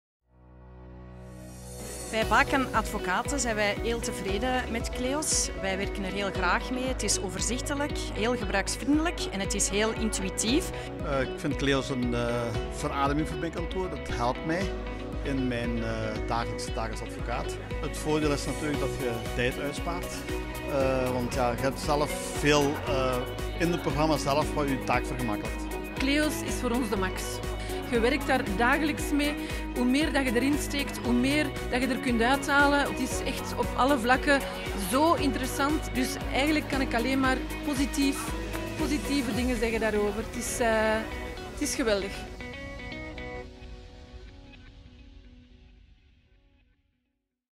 Bekijk welke impact Kleos heeft voor deze advocaten
Kleos is ontwikkeld voor zowel kleine als grote advocatenkantoren om het dagelijkse werk als advocaat eenvoudiger en efficiënter te maken. In deze video vertellen 3 Kleos gebruikers welke impact Kleos heeft op hun kantoor en hun dagelijkse werking.